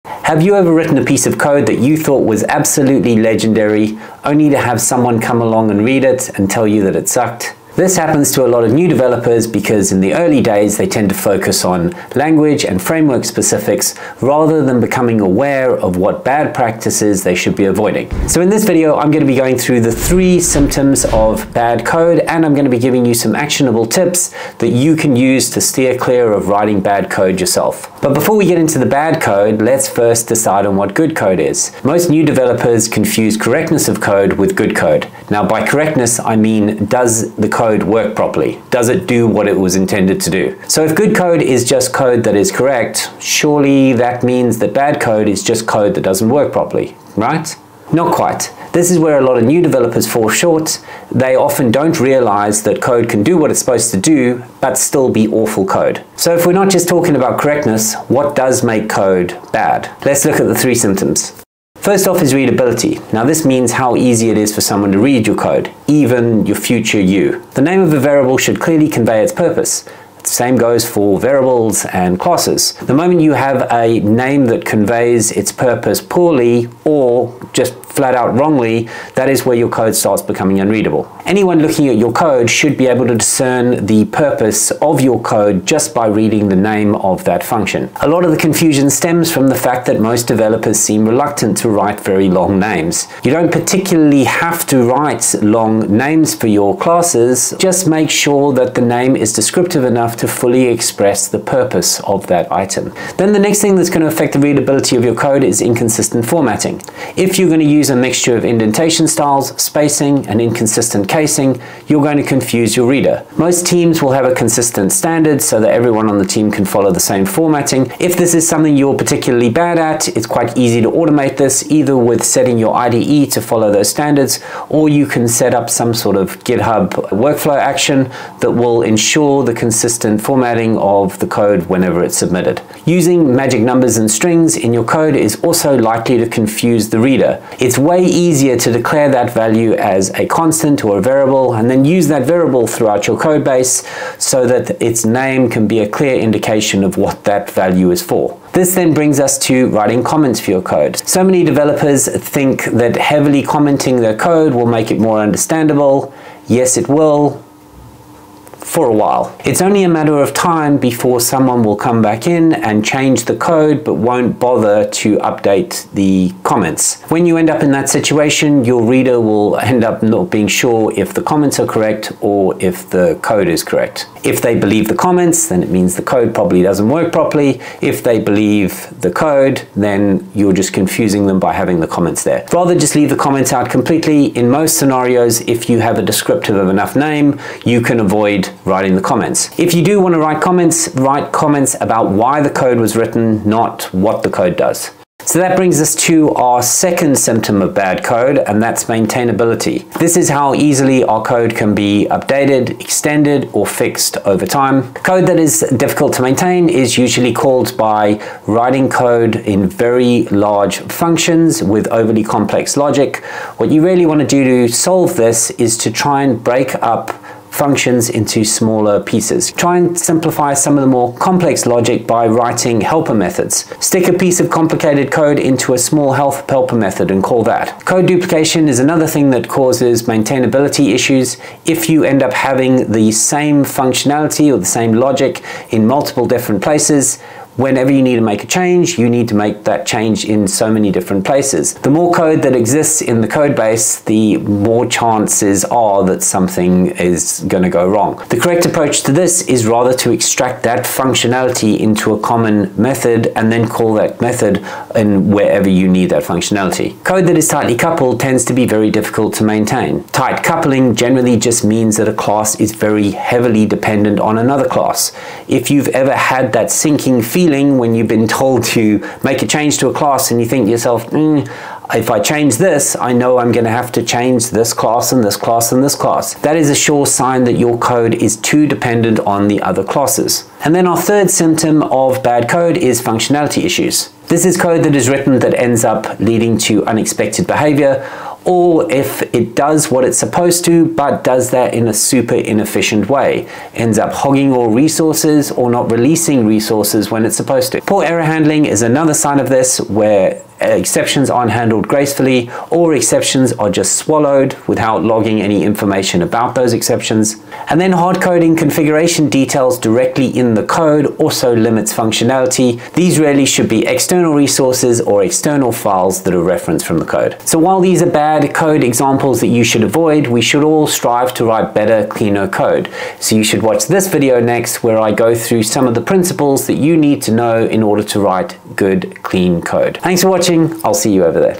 99% of Junior Developers Don’t Know what BAD Code is
Most new developers get caught up in learning the specifics of a new language or framework in their early days of software development, instead of focusing of bad practices when it comes to writing code. In this video we talk about the 3 main symptoms of bad code, and how to fix them (with examples in c# csharp and .net).

📖 Chapters
00:00 - Start
01:01 - Symptom 1
03:15 - Symptom 2
04:40 - Symptom 3